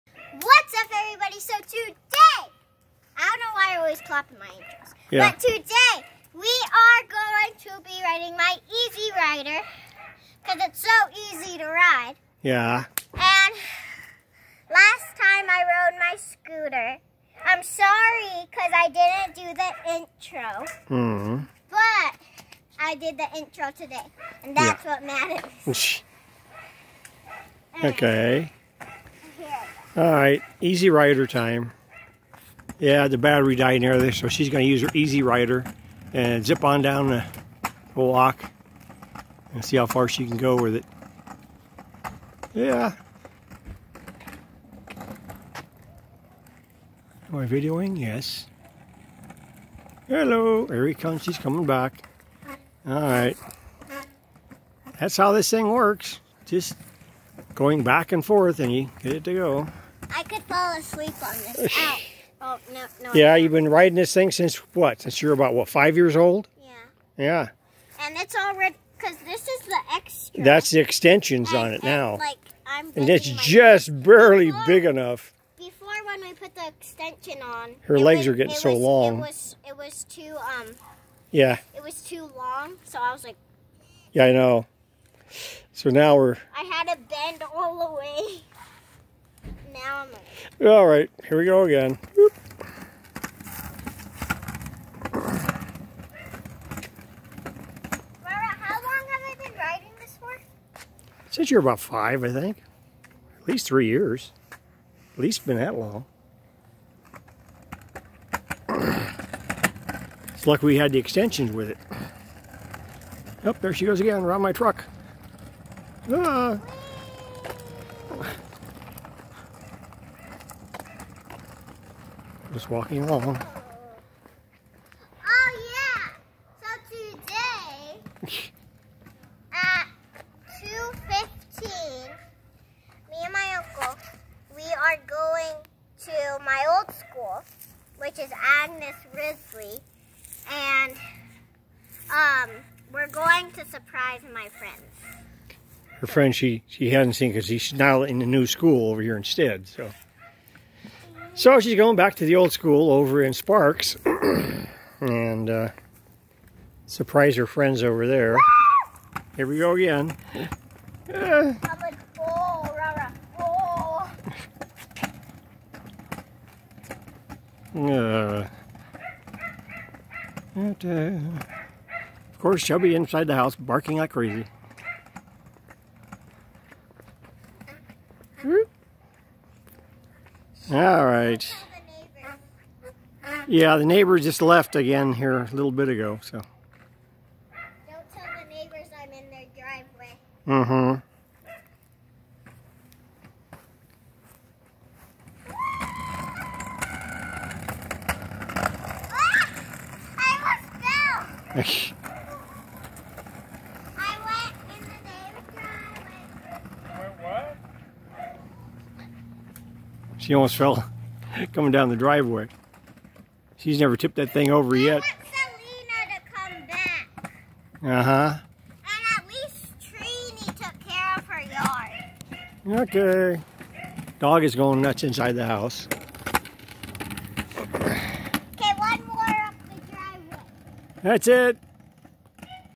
Aniyah on her EZ Rider
Aniyah riding up and down the side walk in my front yard and in the street